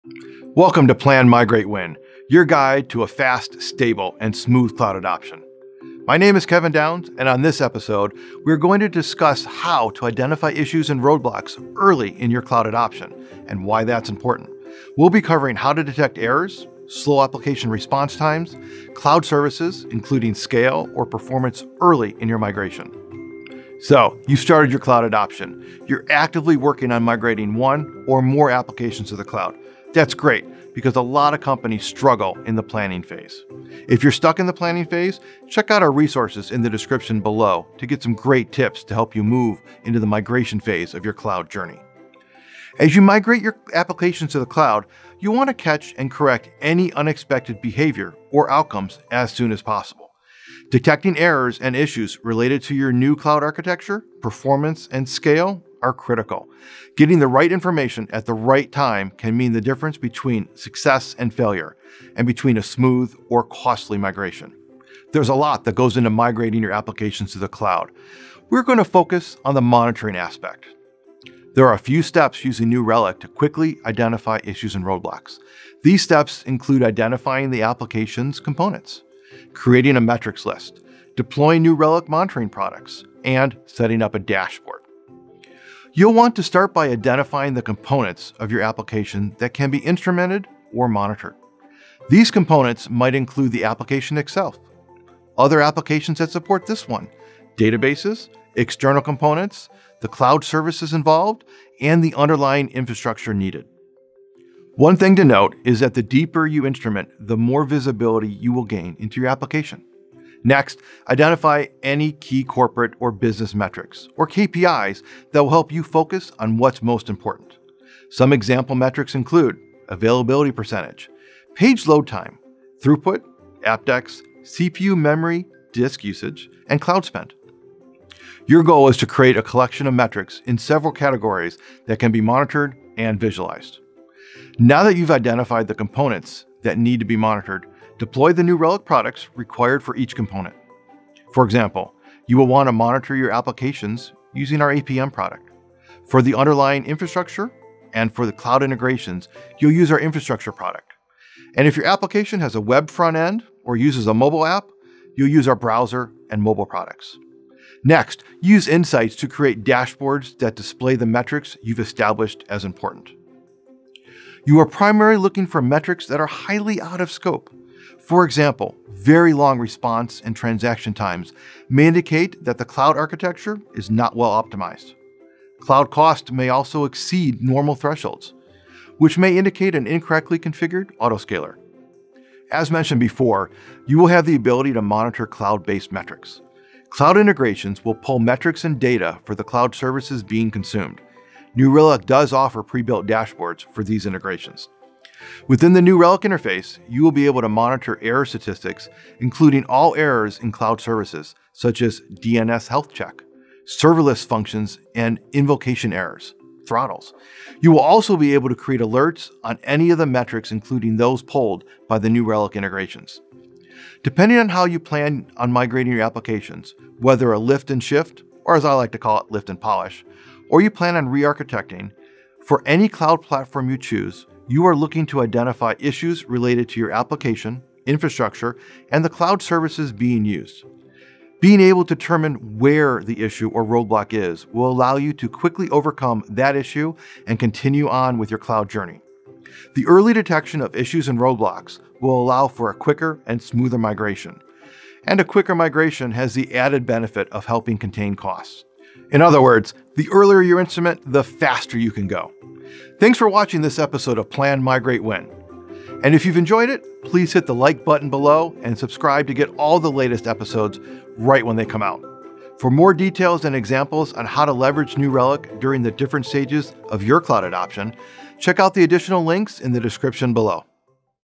Identifying Issues and Roadblocks
In this episode of "Plan, Migrate, Win," Kevin Downs walks us through catching any unexpected behavior or outcomes as soon as possible during your migration to the cloud. Detecting errors and issues related to your new cloud architecture, performance, and scale is critical—getting the right information at the right time can be the difference between success and failure.